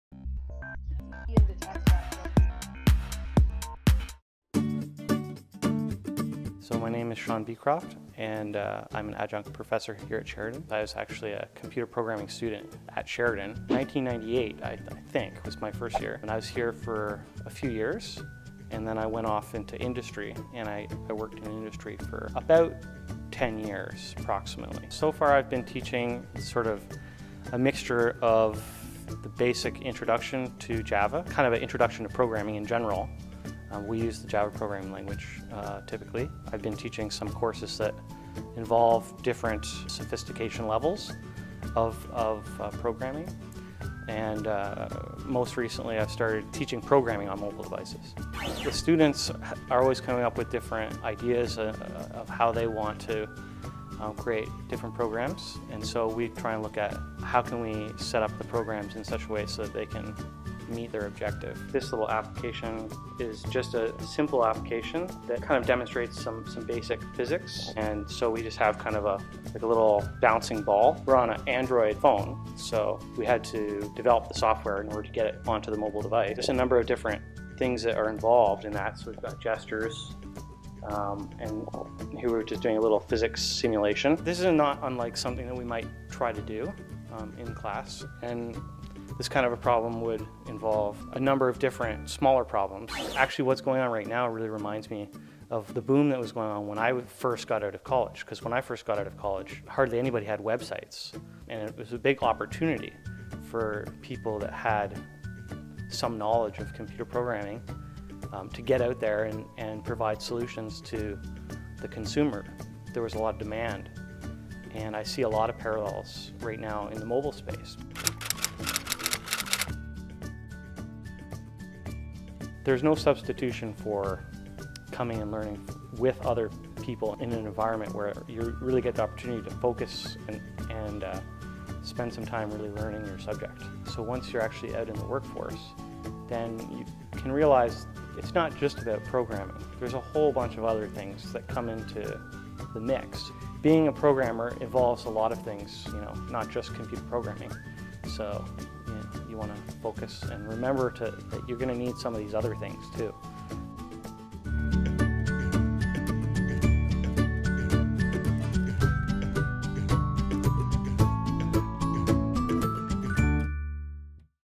Tech Talk - Programming at Sheridan College
Sean Beecroft is an adjunct professor at Sheridan College who teaches computer programming and mobile technologies. Sean shares some wisdom about the current industry as well as showcasing one of the basic mobile programs students can work on at Sheridan.